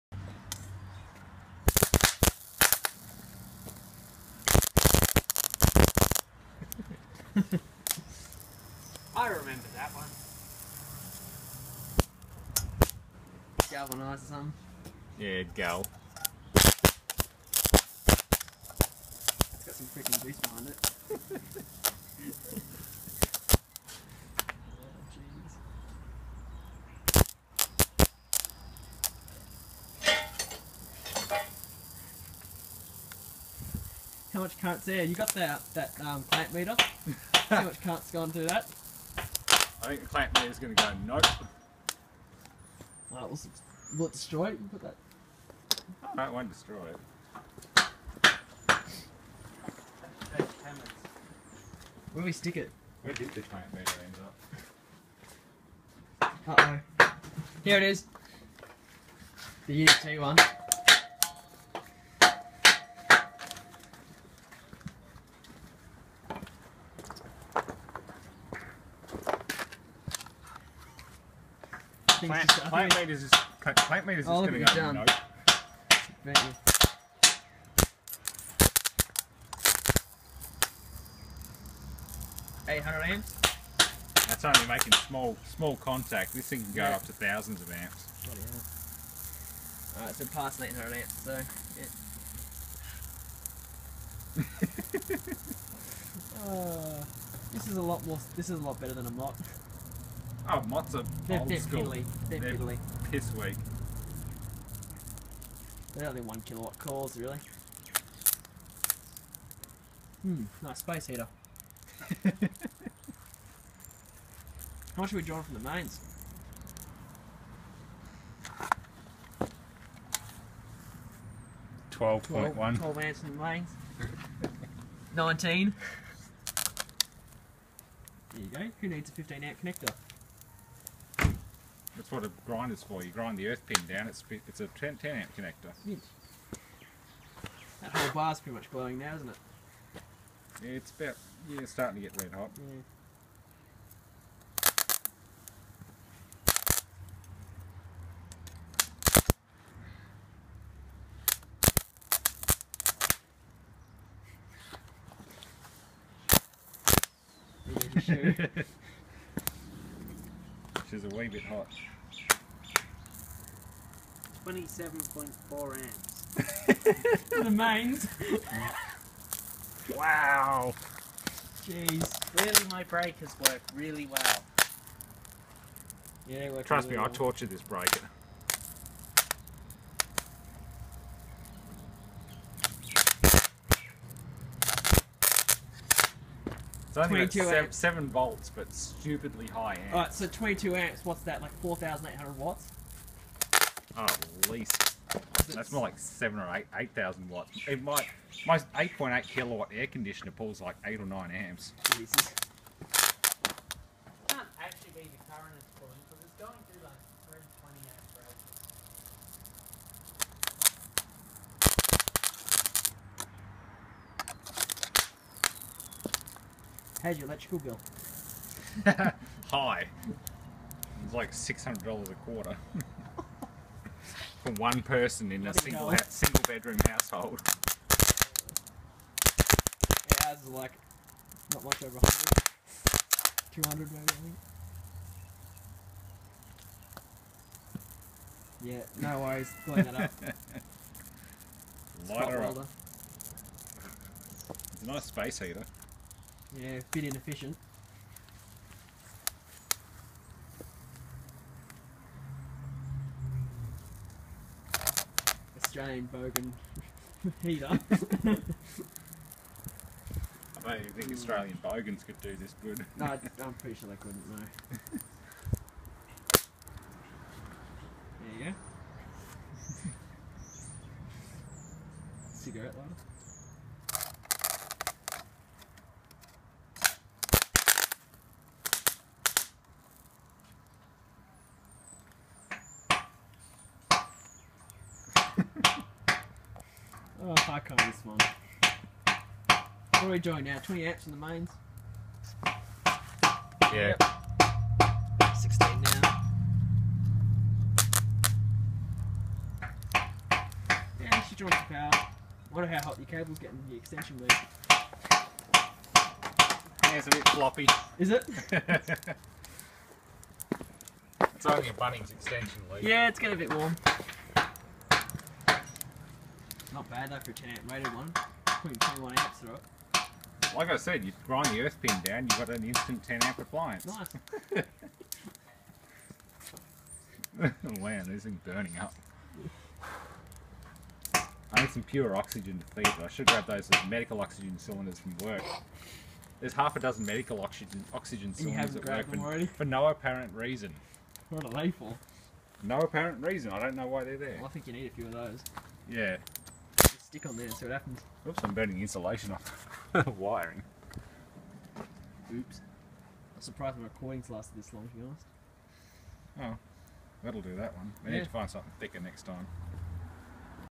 High Current Madness with Aussie50
Playing with a Homemade High current transformer that Ed made. Simply an arc welder transformer rewound with railway cable to give thousands of amps.